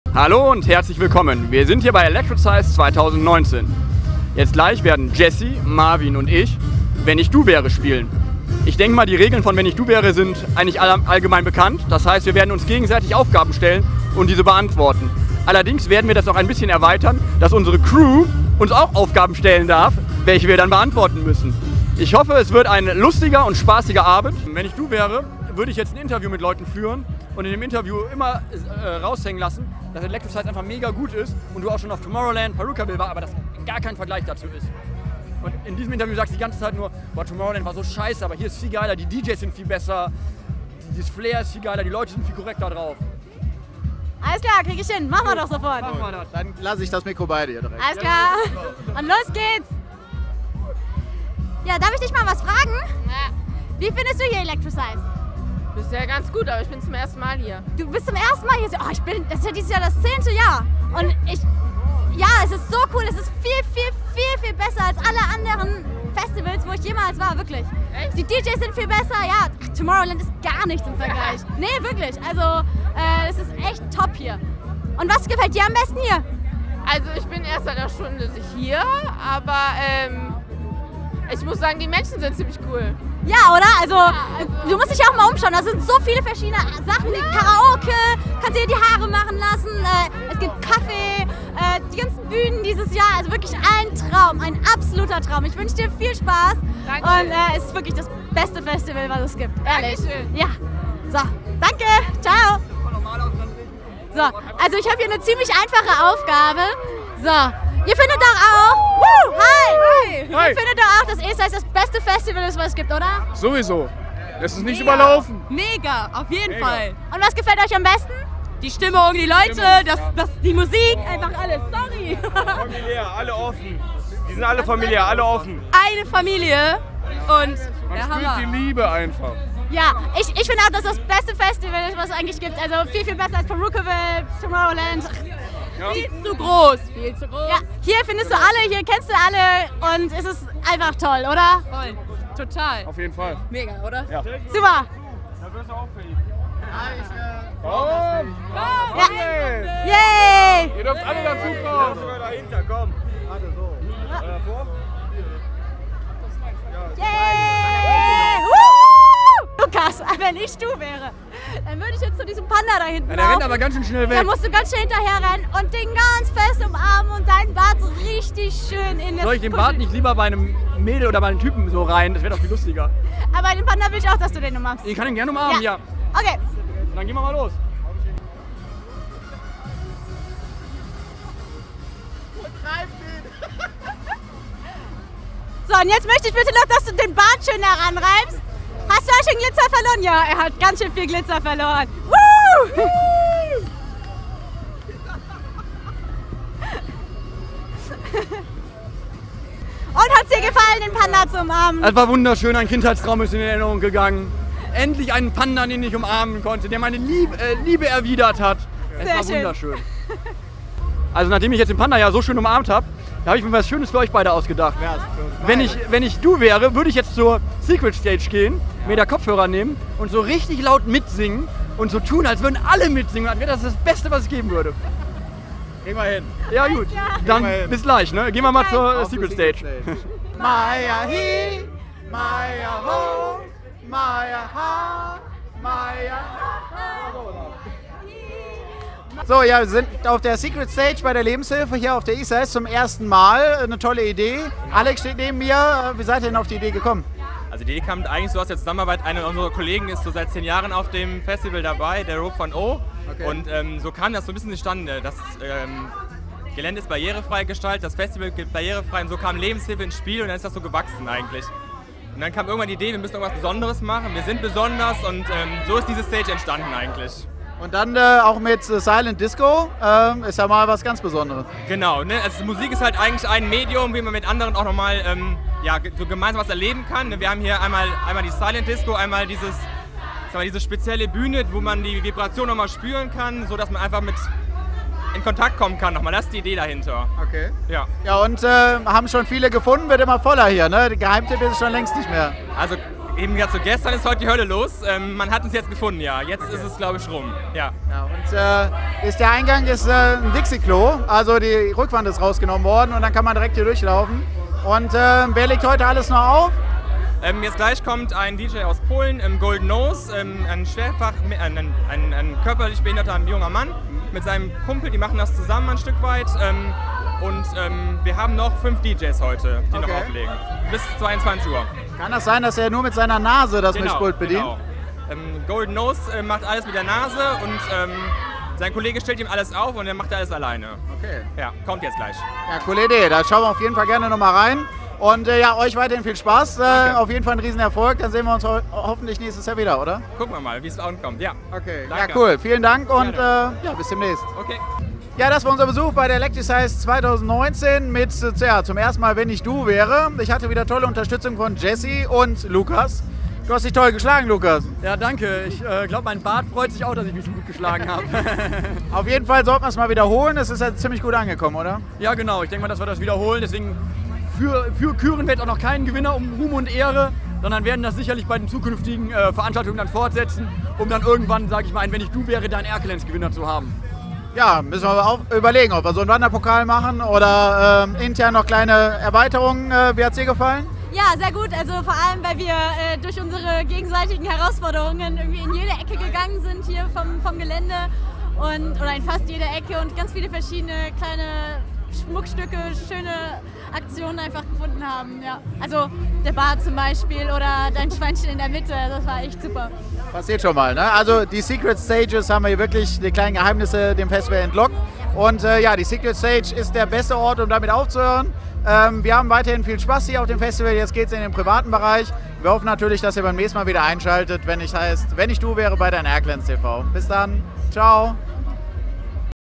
Dein-Erkelenz.tv: Wenn ich du wäre... Teil 3 - ESIZE19
Auch in diesem Jahr waren wir natürlich wieder mit verschiedenen Aktionen auf der Electrisize unterwegs. nun folgen nach und nach die Videos 🤩 Weiter geht's mit dem dritten Teil von WennIchDuWäre - viel Spaß🥳